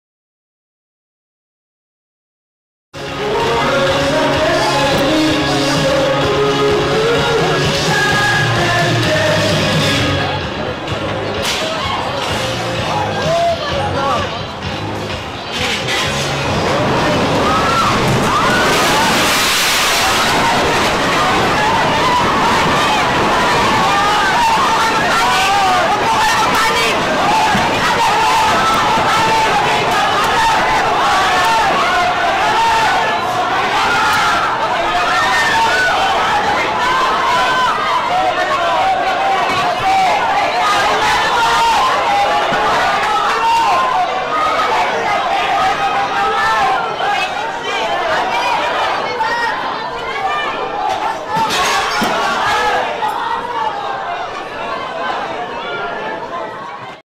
Momen Bahagia Berubah Menjadi Mencekam Saat Tornado Hantam Pesta Pernikahan
Pesta pernikahan adalah momen bahagia dalam hidup

Tapi bagaimana jika pesta pernikahan berujung dengan kengerian